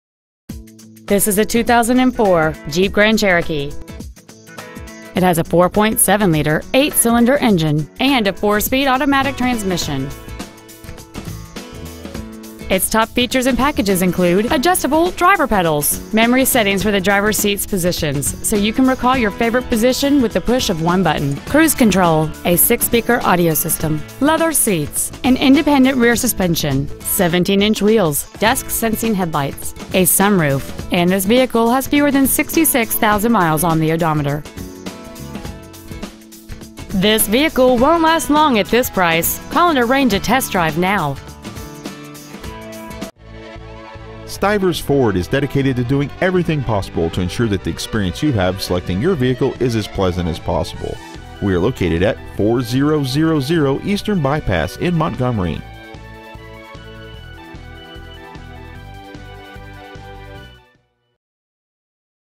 2004 JEEP GRAND CHEROKEE AL
Year : 2004
 Make : JEEP
 Model : GRAND CHEROKEE
 Engine : 4.7L V8
 Trans . : AUTO 4SPD
 Exterior : RED
 Miles : 65,618
 Interior : DARK SLATE GRAY
 Stock : CA54984A

 4000 Eastern Bypass
  , AL 36116